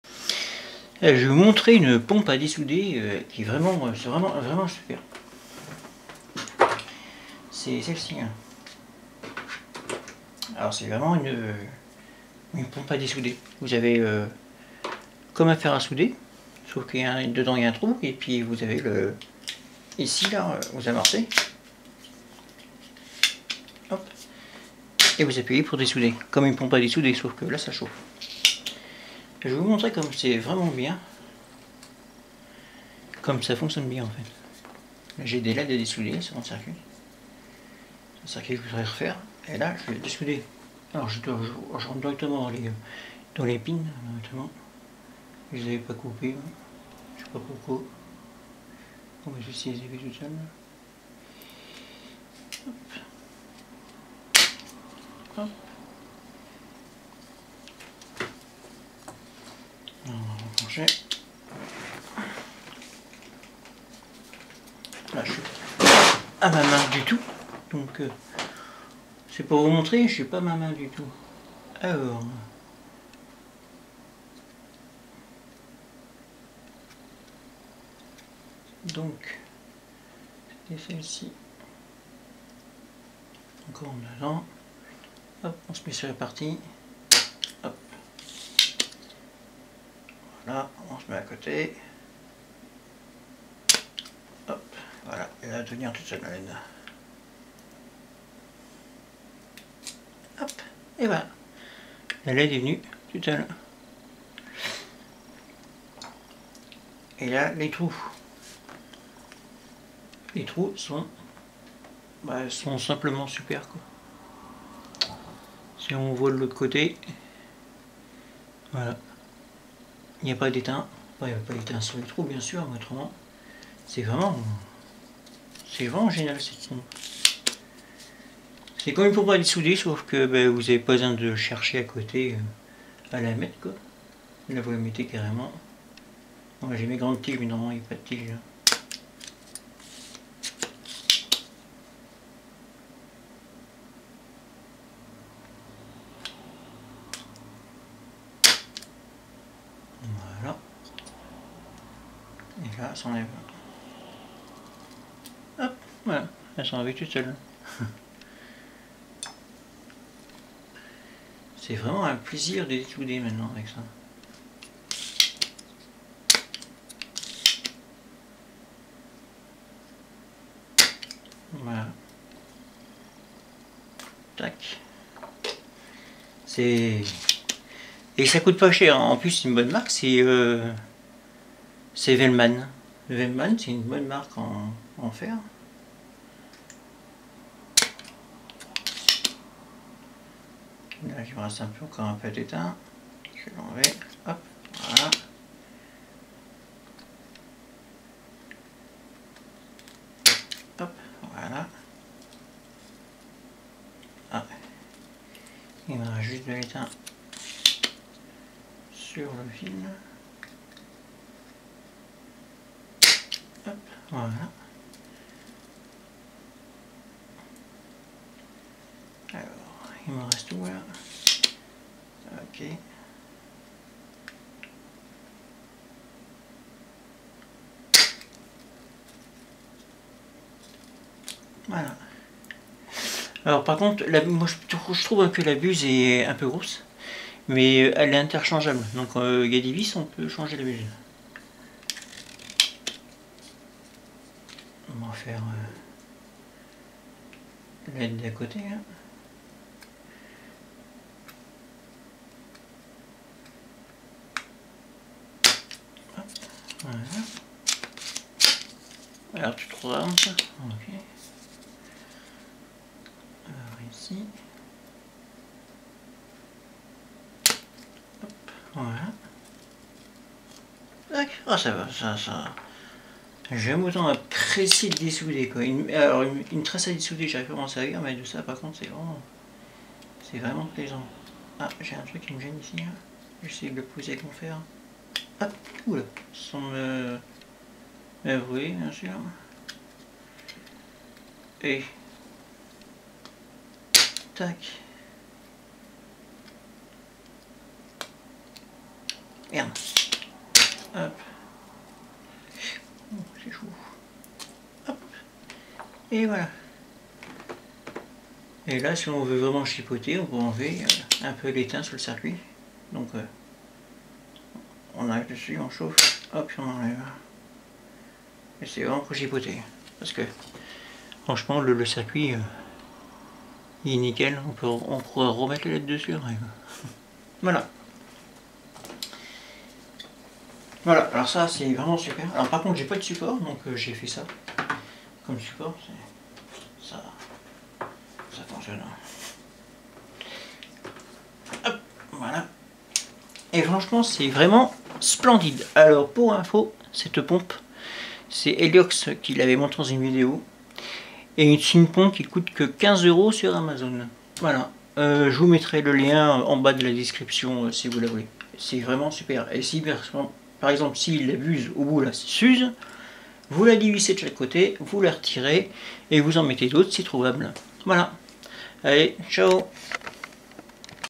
Velleman VTDESOL3 Pompe à dessouder
Présentation de la pompe à dessouder de Velleman qui est vraiment génial!!
pompe à dessouder
buse de rechange